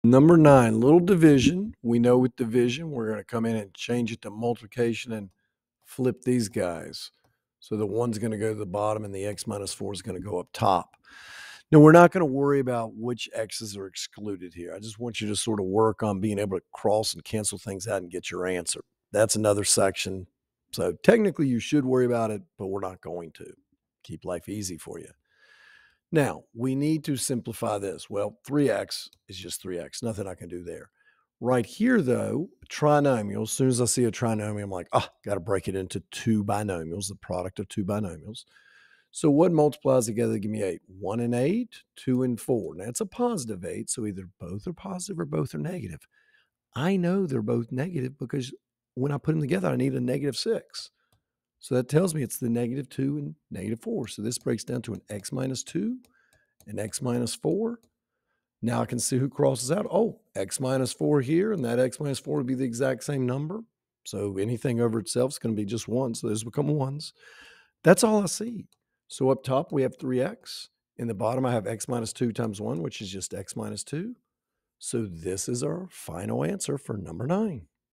Algebra 2 9-4 #9
Division of Rational Functions requiring factoring